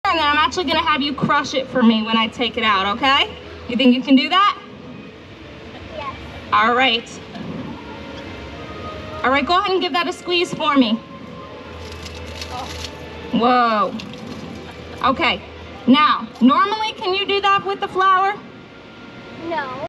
Aurelia Crushes Liquid Nitrogen Flower!
Aurelia got to crush a flower that was placed in liquid nitrogen during a demonstration at the Kennedy Space Center.